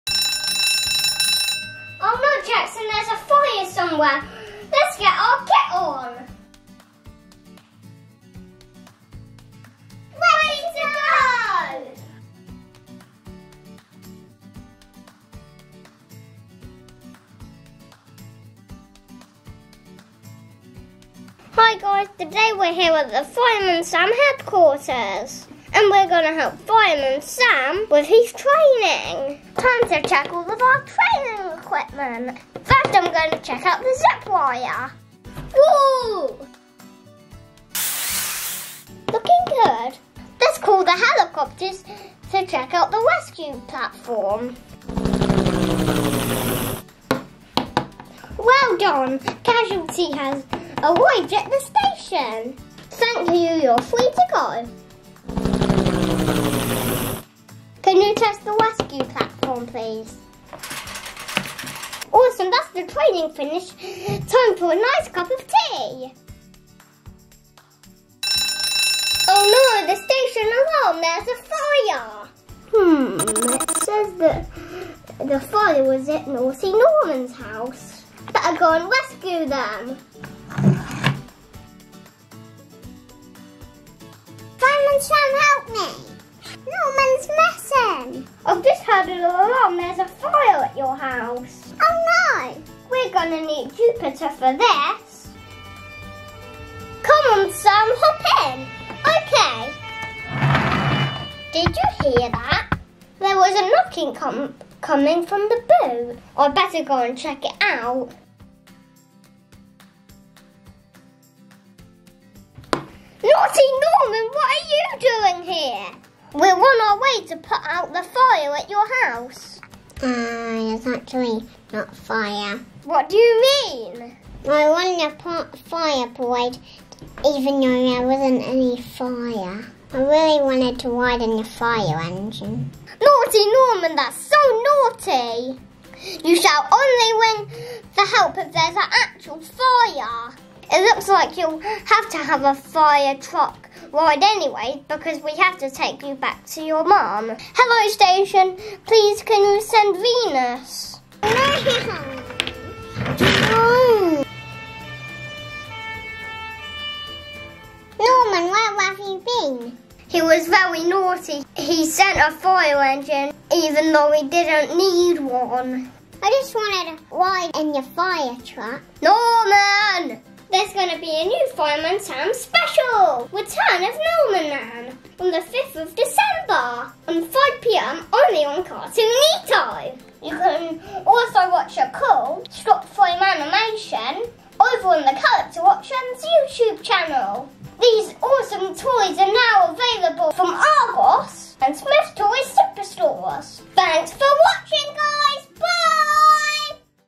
Fireman Sam Fire Rescue Centre
(AD)
Today we check out the new Fireman Sam Rescue Centre from @CharacterOnlineToys

Open up the feature-packed Fire Station to reveal the fire fighters' headquarters. There’s a working zipwire, platform with pulley and training dolly, control centre unit, plus more! Also includes articulated Fireman Sam figure. Use the carry handle to take Sam and the Rescue Centre to the next mission! Scaled for use with other Little Character Fireman Sam toys.